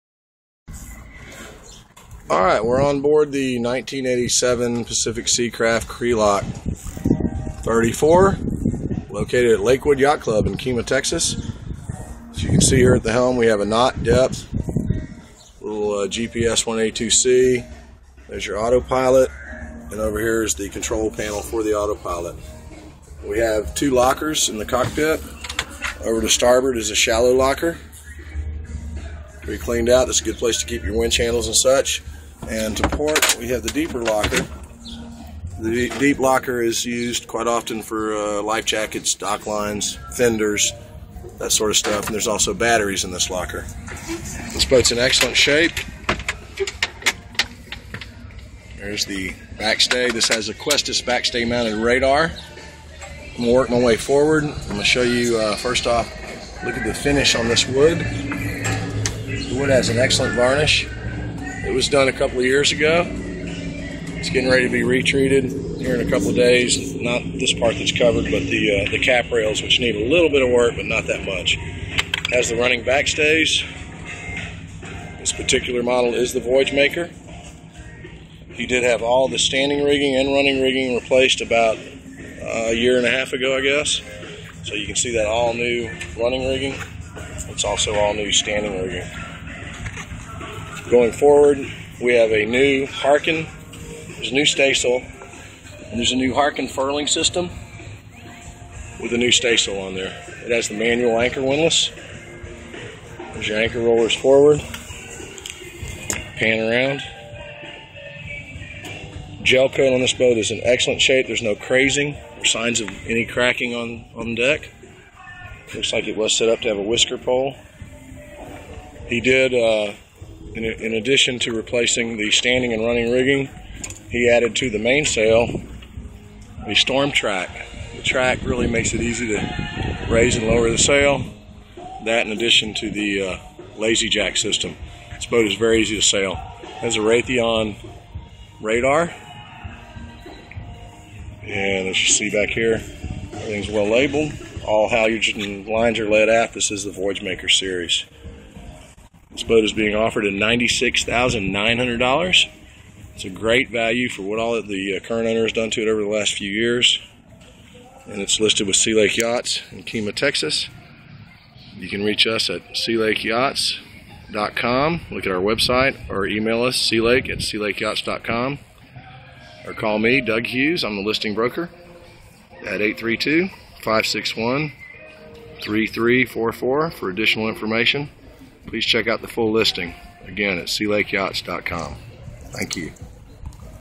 1987 Crealock 34 Listed with Sea Lake Yachts asking $96,900.
This is a tour of the deck on this beautiful Pacific Seacraft Crealock 34 Listed with Sea Lake Yachts.